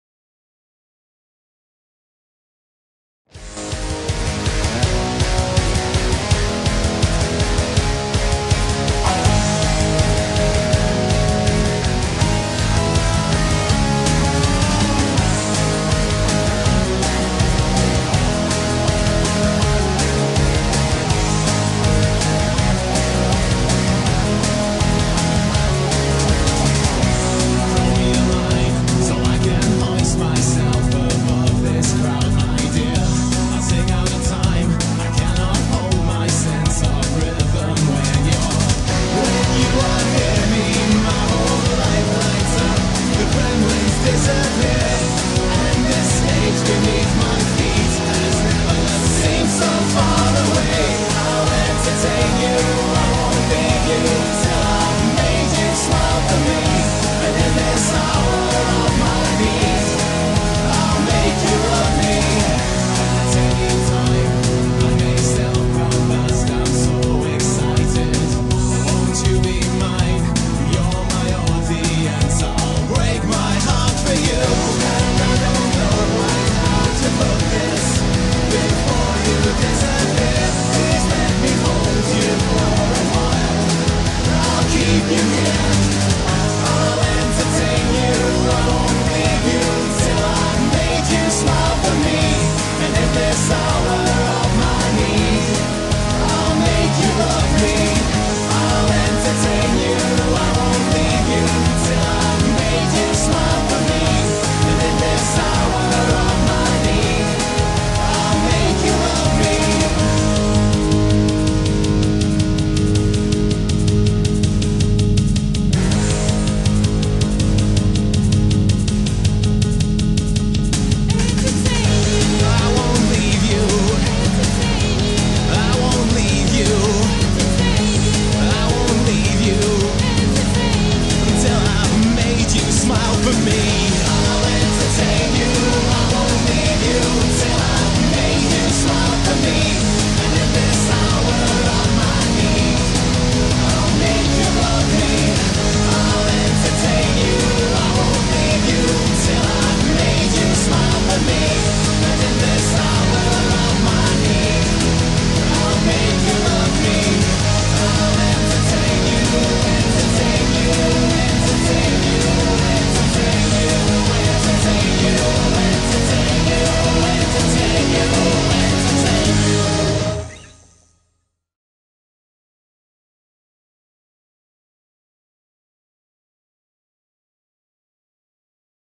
Gotham Slideshow - Screaming Banshee Aircrew
A slideshow from our appearance at Gotham festival, Islington Academy, London in 2006.